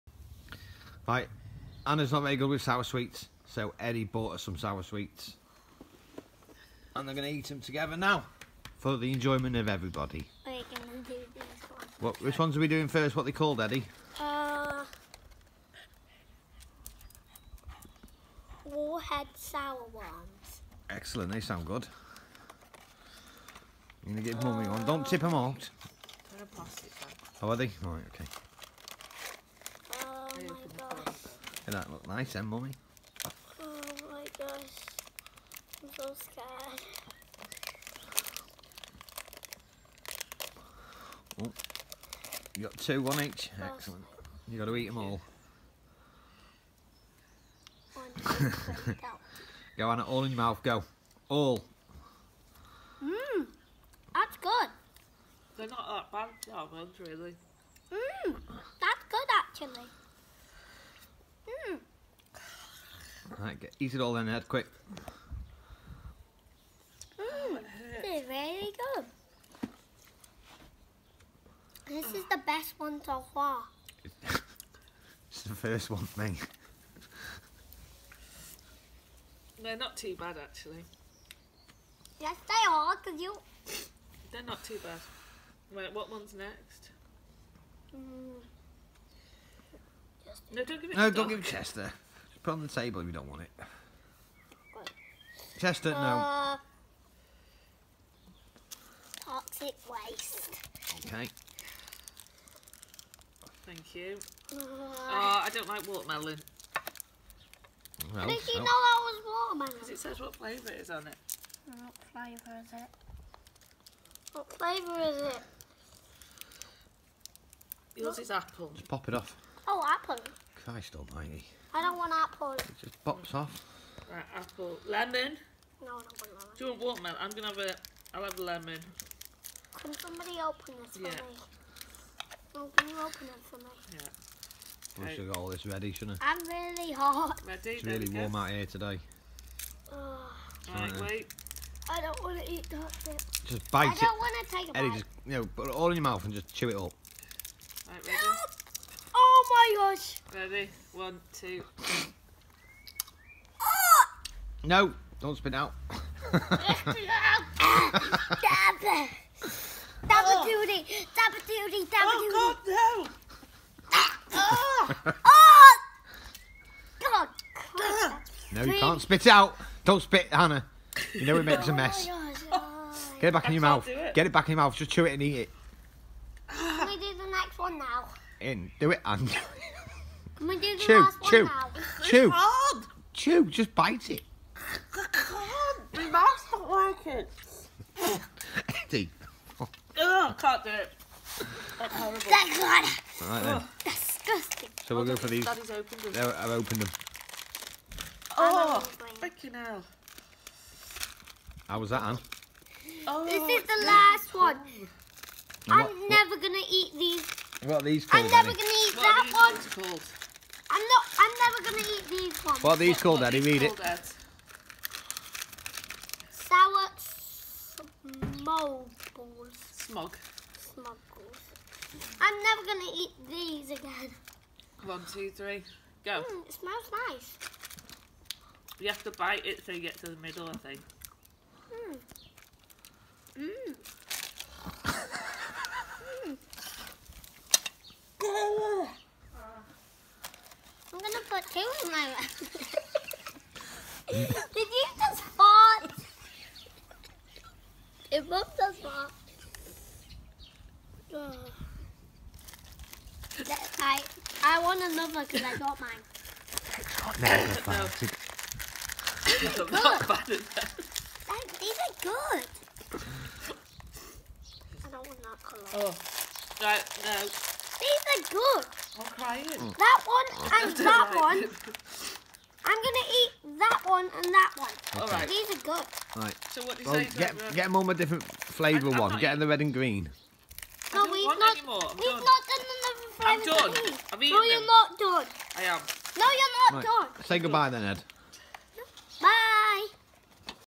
Eddie eats sour sweets
Eddie wanted to make a YouTube video with mummy where they ate sour sweets